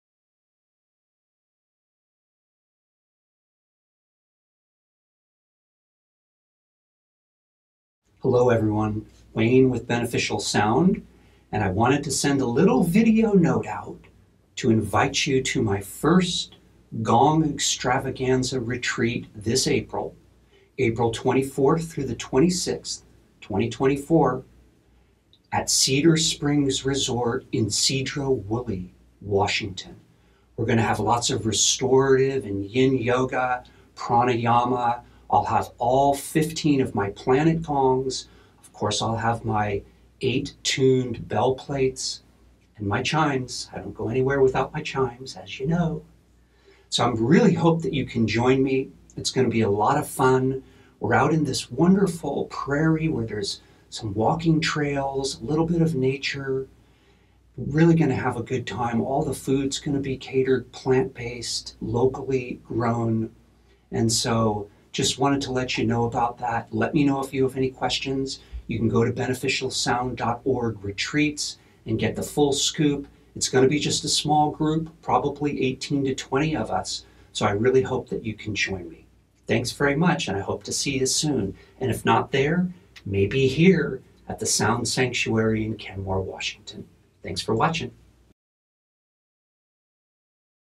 The Higher Octave Retreat Invite
Short video invitation to the Higher Octave.
Raise Your Personal Vibration with a blissful weekend of Movement, Breathwork, Nature and Sound!

Spring Retreat 2024
April 26th-28th
Cedar Springs Lodge
31463 Barben Rd.